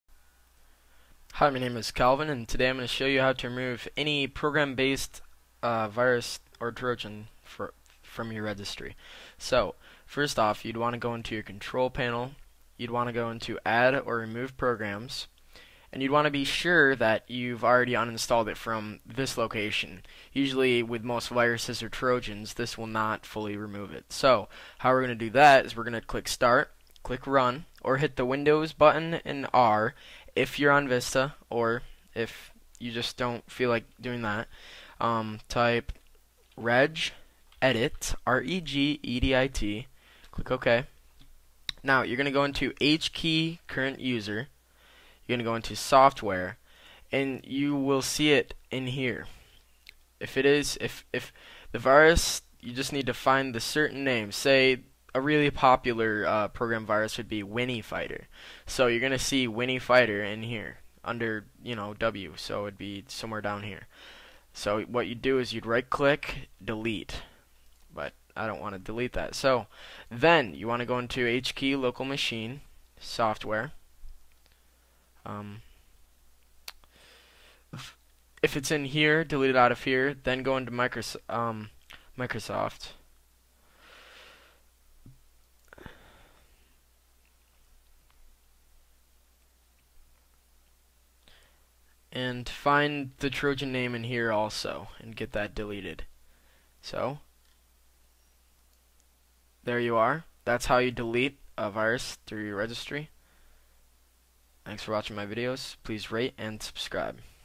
How to remove any Virus from your pc
how to remove any program based virus from your computer with regedit.